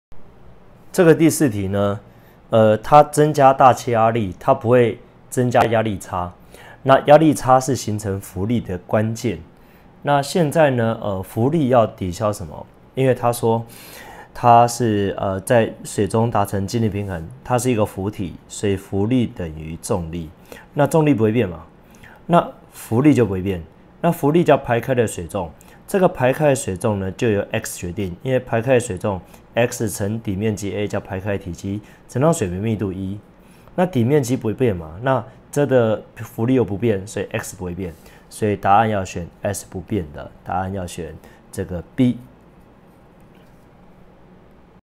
104台中一中資優班04
104台中一中數理資優班物理詳解
104台中一中數理資優班考古題

國立臺中第一高級中學 104 學年度 學術性向資賦優異【數理類】學生入班鑑定安置計畫 自然學科能力評量(物理科) 試題卷詳解